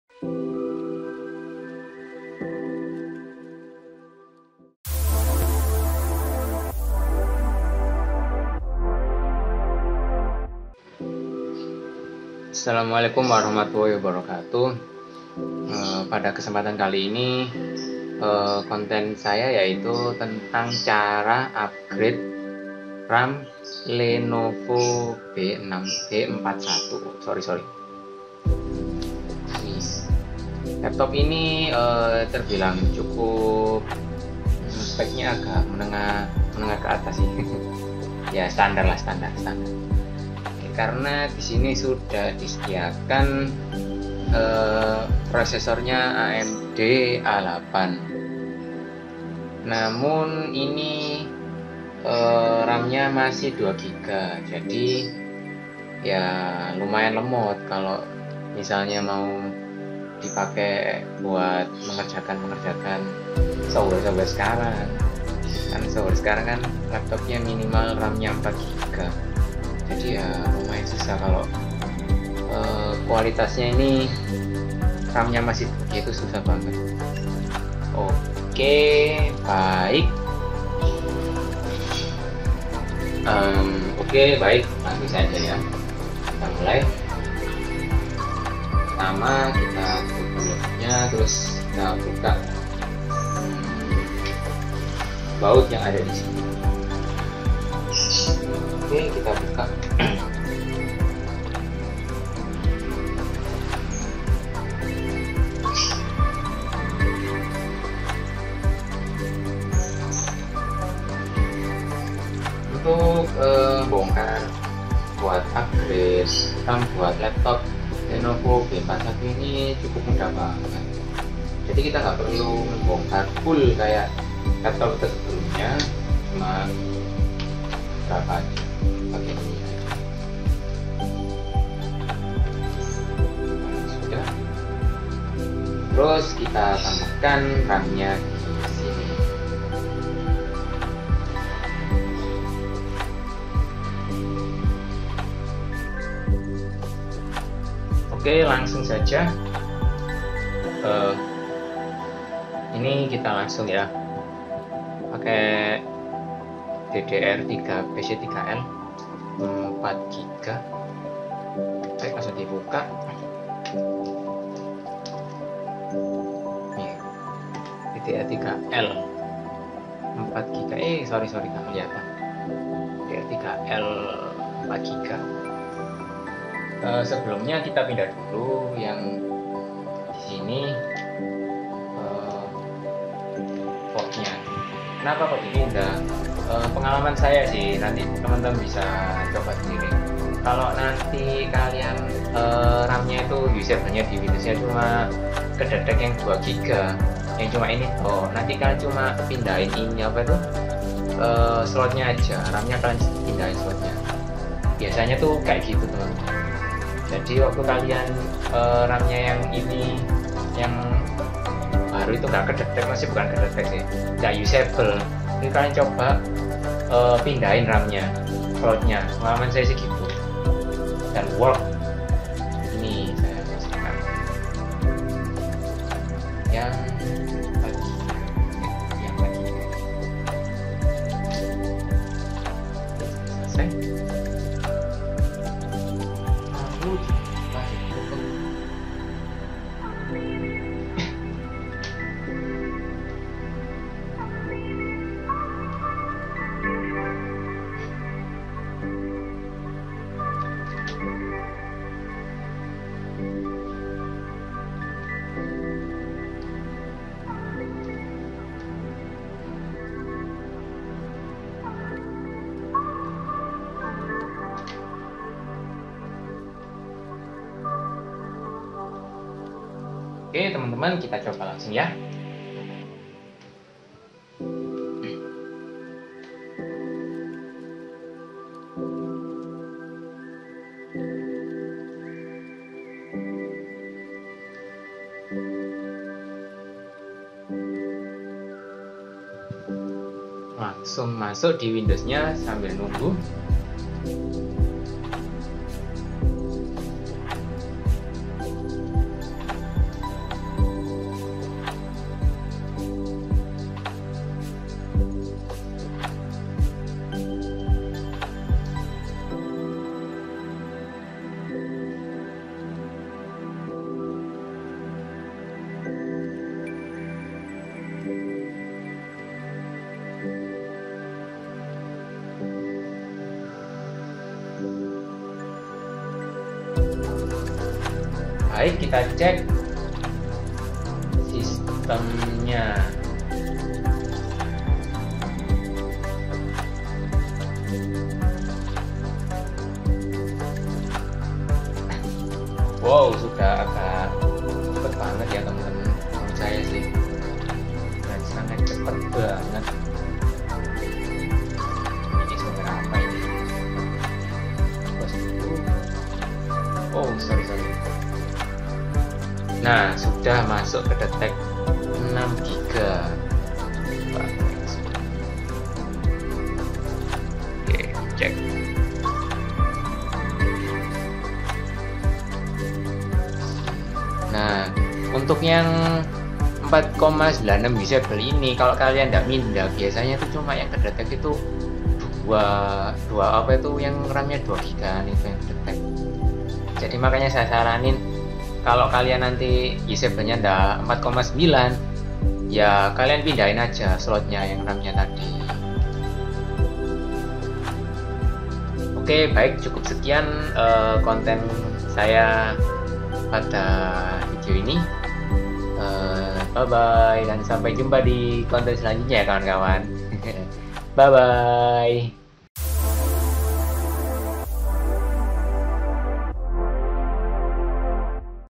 Cara Upgrade RAM Laptop Lenovo B41
Pada Kesempatan kali ini kebetulan ada laptop client yang ingin minta di upgrade ke saya karena laptopnya hang banget sebab ya gitu RAMnya masih 2 gb alhasil untuk kinerjanya sendiri lumayan lag banget, dibuat untuk membuka firefox saja loadingnya lumayan lama.